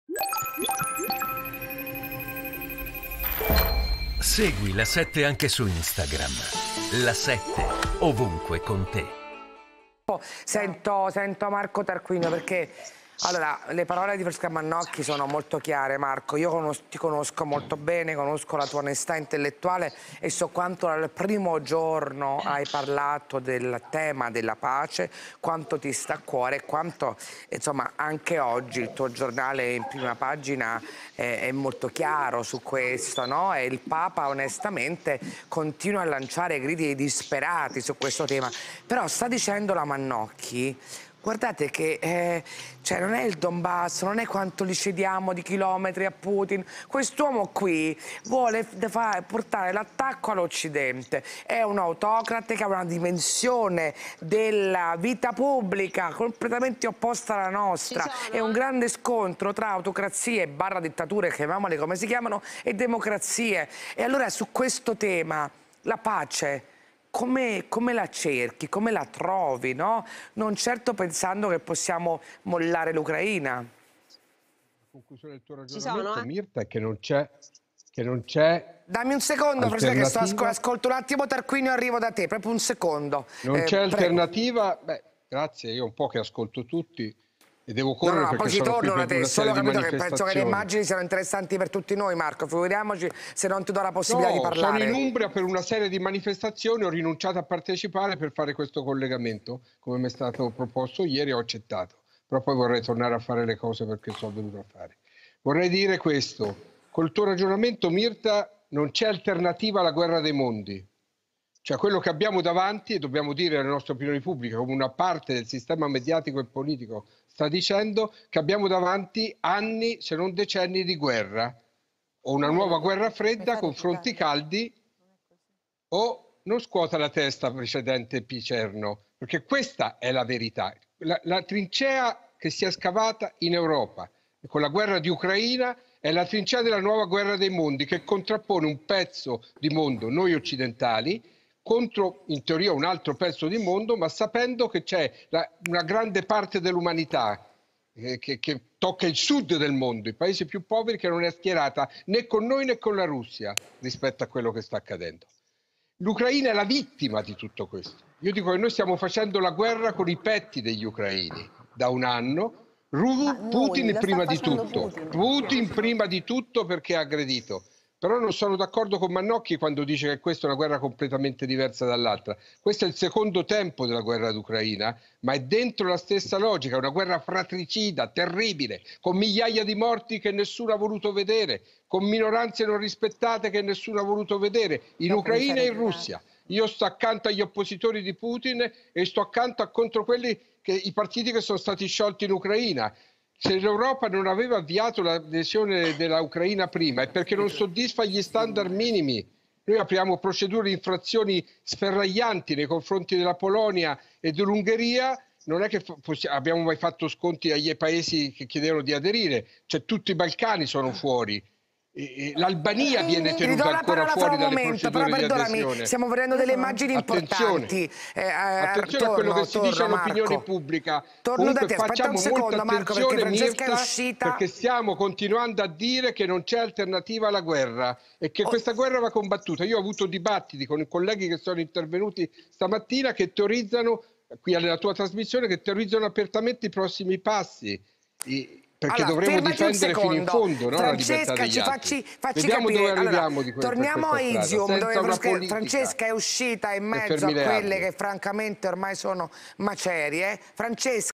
Marco Tarquinio a Myrta Merlino: "Ho rinunciato a una serie di manifestazioni per fare questo ...
Marco Tarquinio (Direttore de L'Avvenire): "Sono ad Assisi per una serie di manifestazioni, ho rinunciato a partecipare per fare questo collegamento". Myrta Merlino: "Figuriamoci se non ti do la possibilità di parlare".